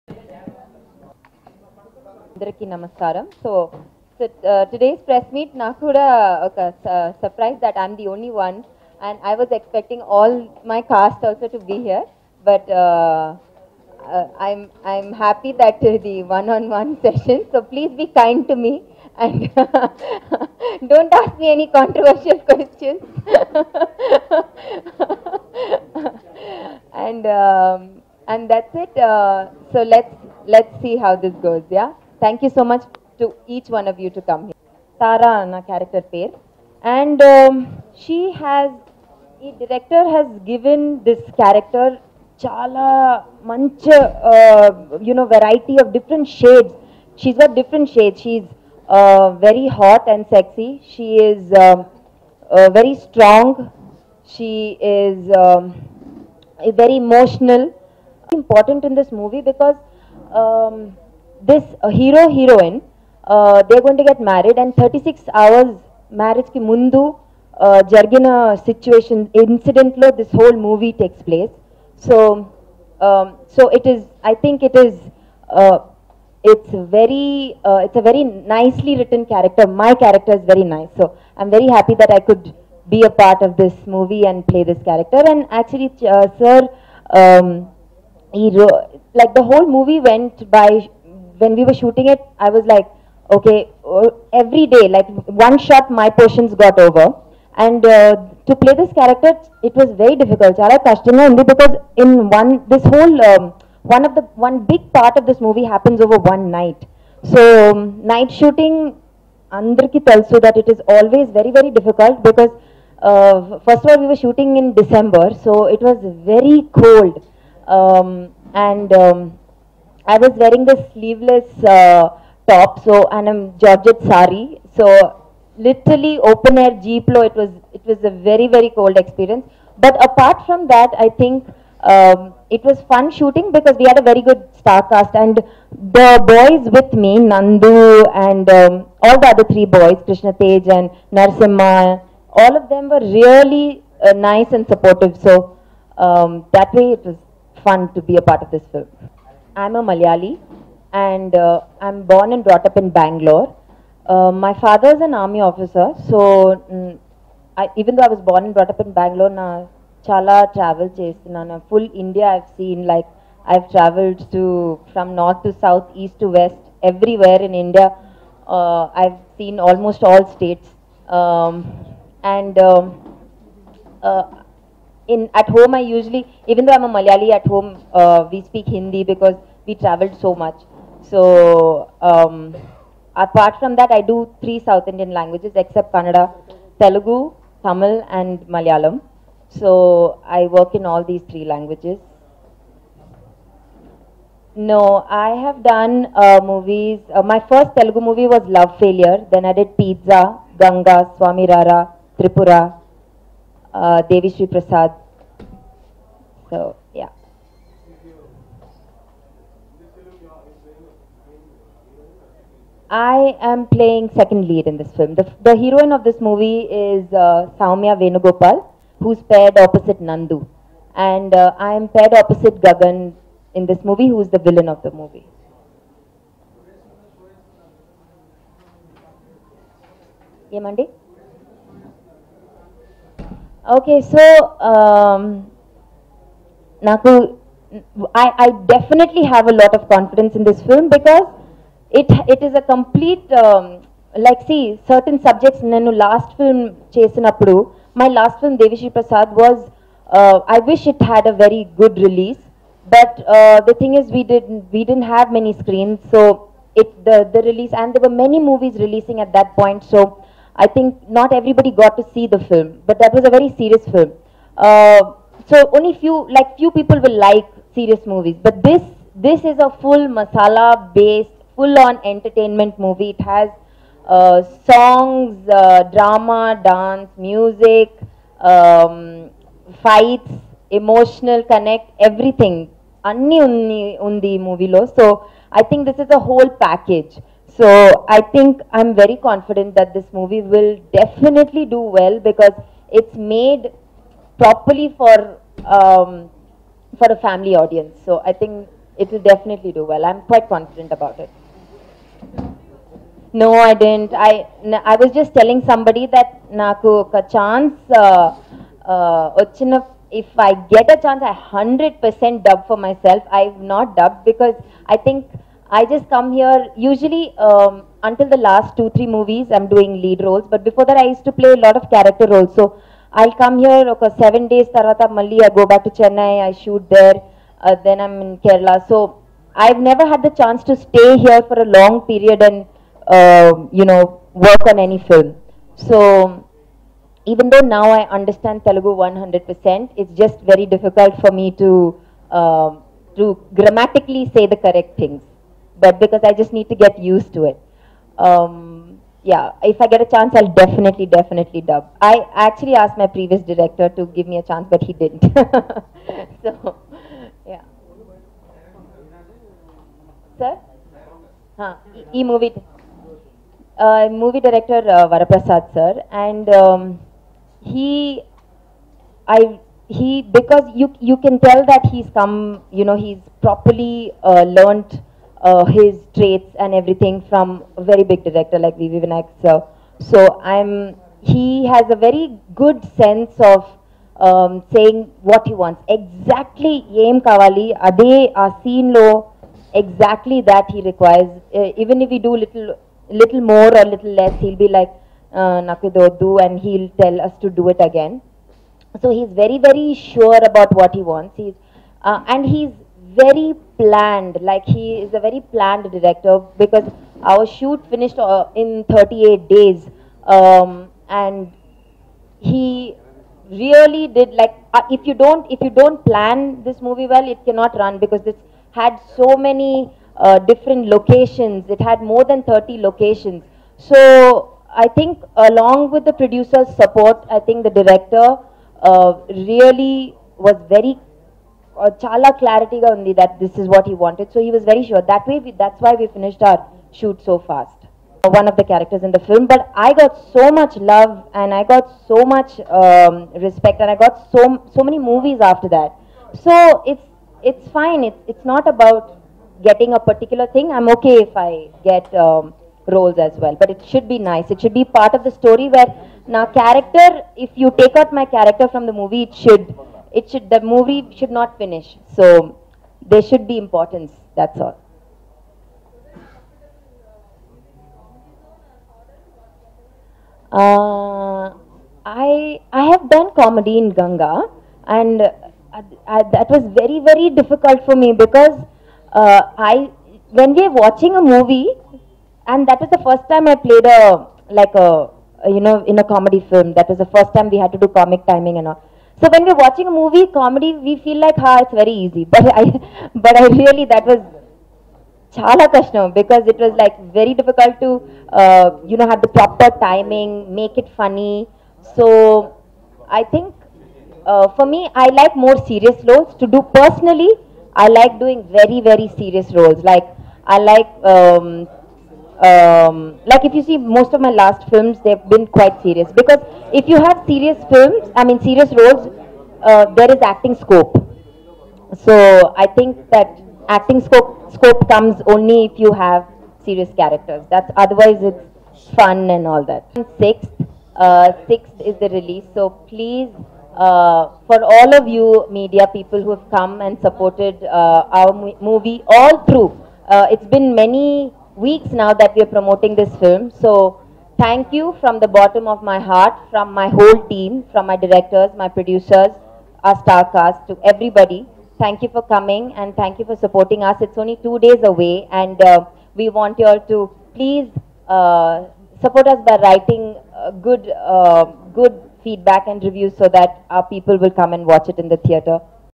Pooja Ramachandran Interview about Inthalo Ennenni Vinthalo Movie | Nandu, Sowmya | NTV ENT
Watch Pooja Ramachandran Interview about Inthalo Ennenni Vinthalo Movie | Nandu, Sowmya | NTV ENT